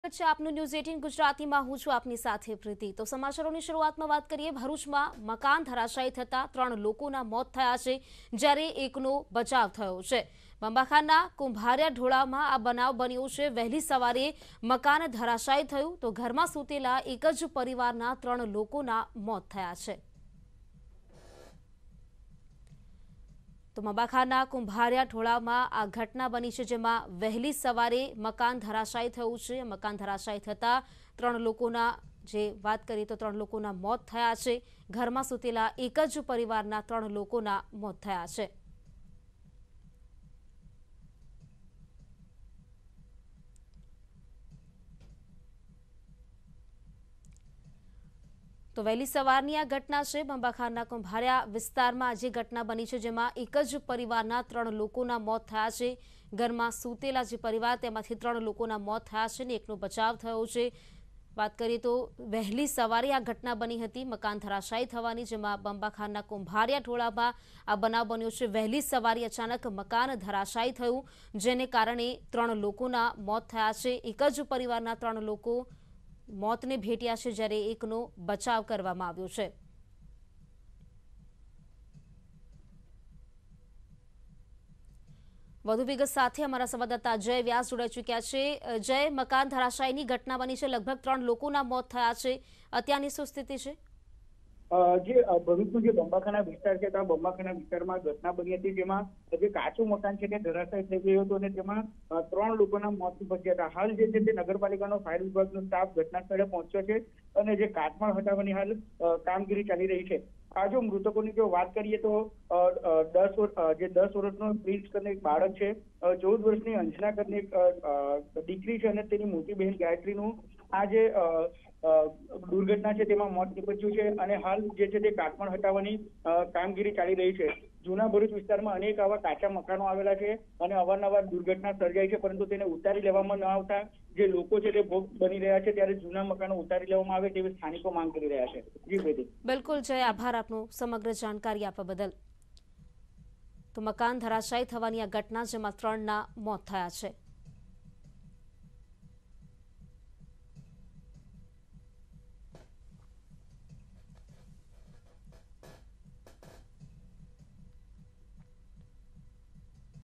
Bharuch માં મકાન ધરાશાયી થતા 3 લોકોના મોત | News18 Gujarati
Bharuch માં મકાન ધરાશાયી થતા 3 લોકોના મોત. સુતેલા એક જ પરિવારના 3 ના મોત, 1 નો બચાવ. બંબાખાનના કુંભારિયા ઢોળાવમાં બન્યો આ બનાવ.